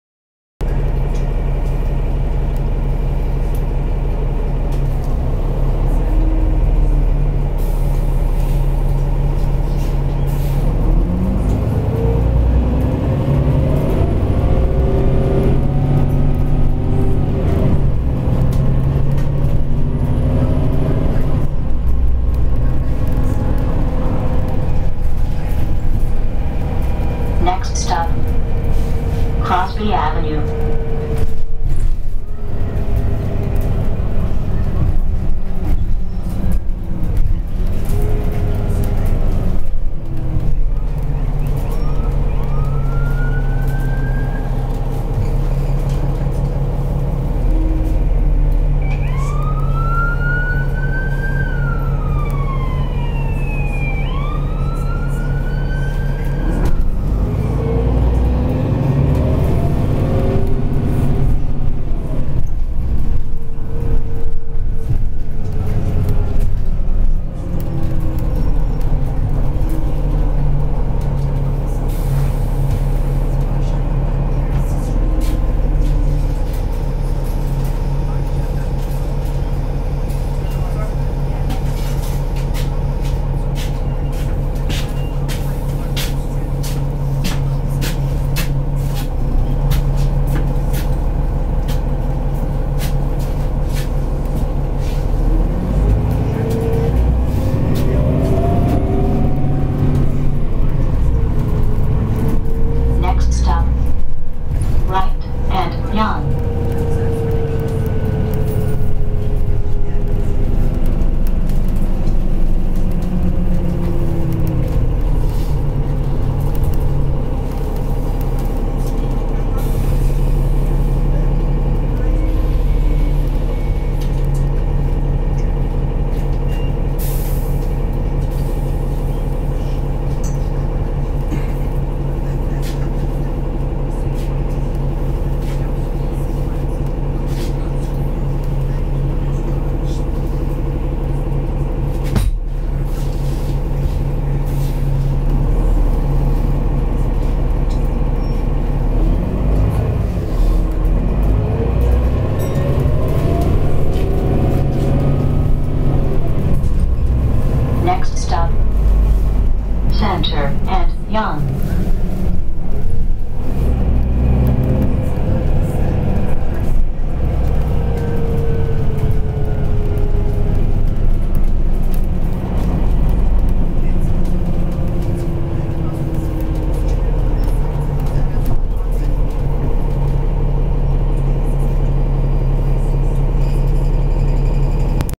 York Region Transit VanHool A330 # 503 (Ex. VIVA # 5112) Part 2
2nd Clip onboard YRT VanHool A330 # 503 on Route 99 Yonge: Taken on 11/07/13